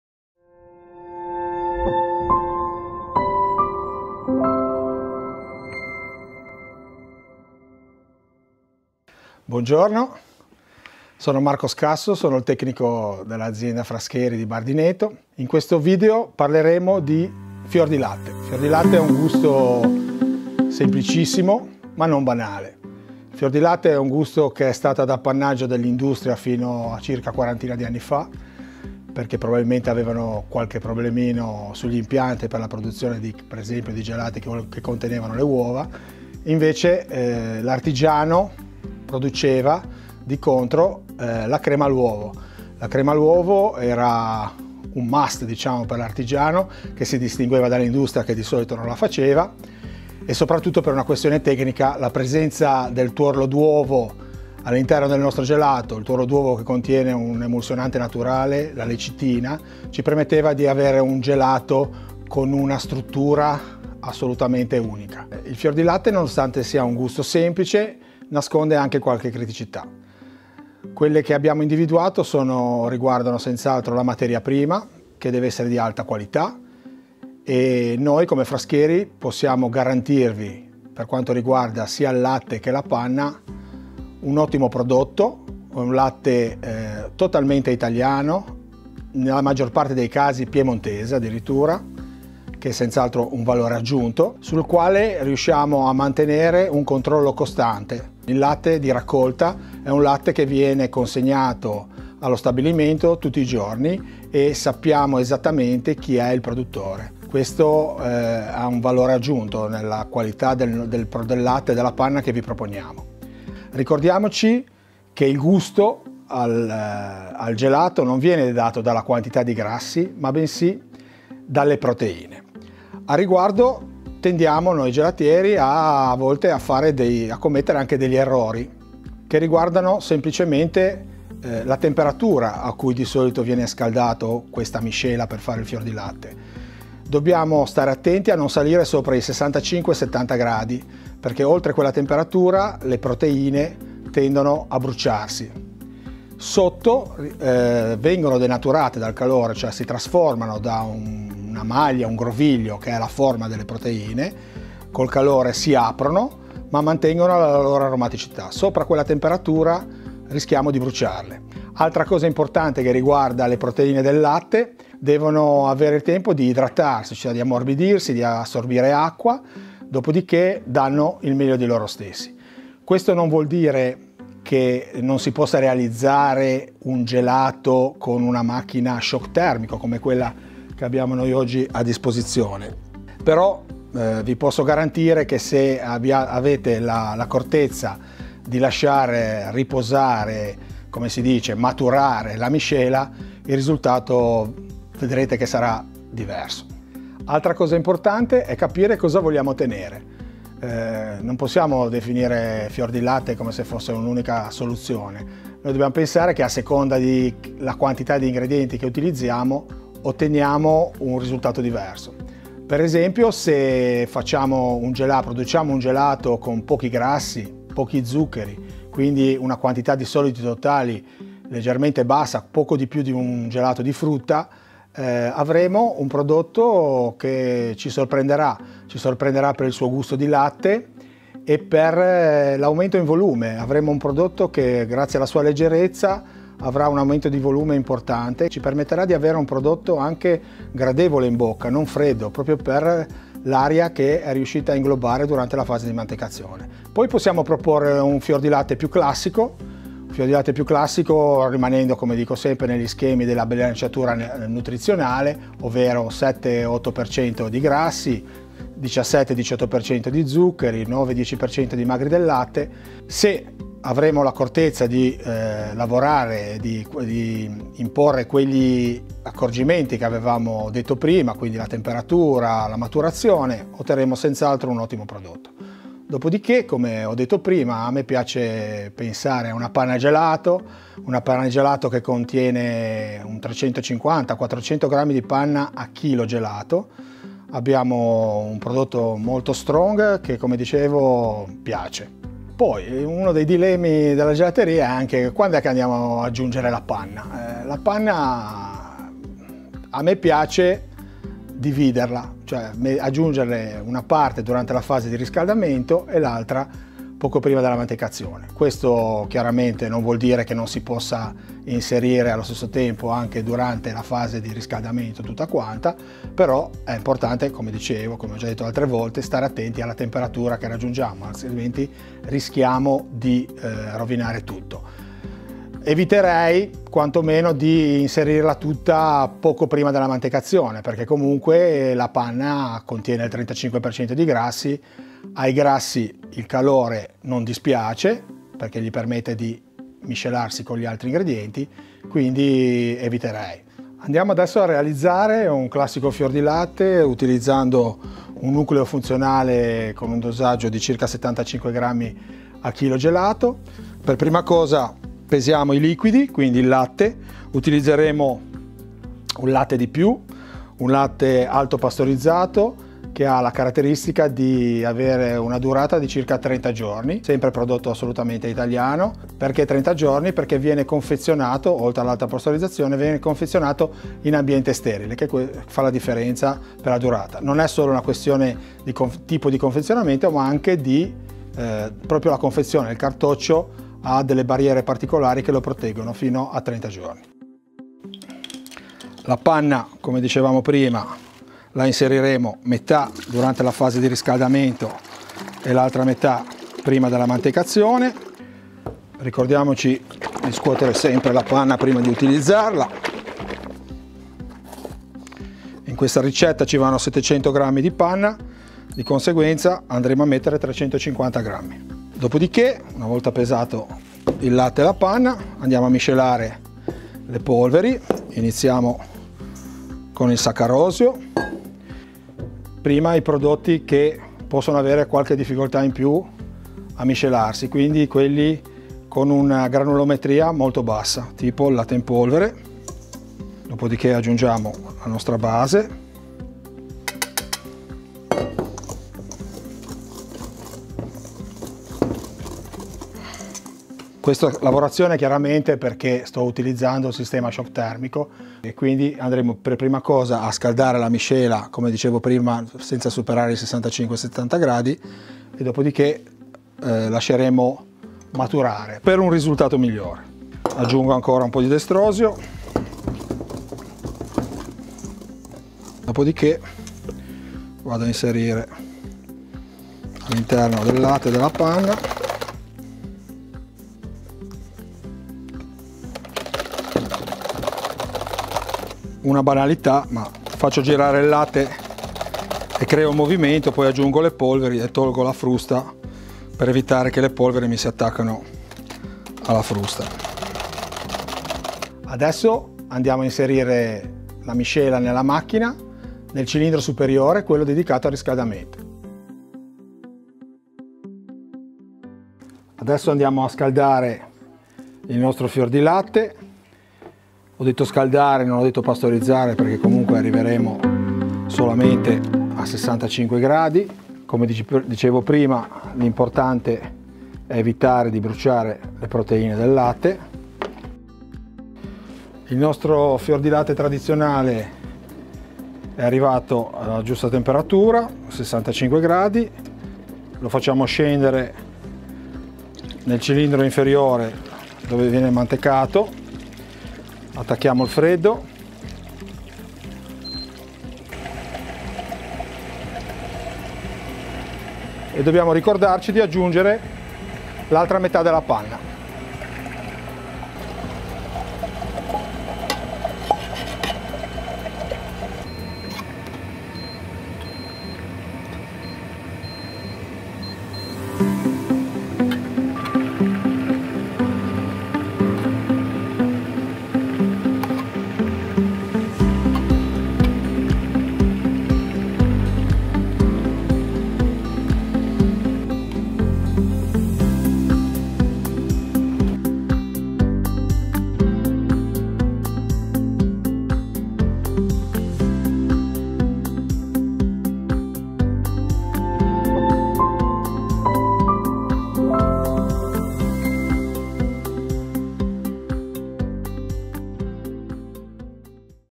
Preparazione del gelato al Fiordilatte
Vuoi provare a realizzare il fiordilatte con i consigli di Marco Scasso?
Guarda la video ricetta con una delle sue versioni preferite e facci sapere cosa ne pensi.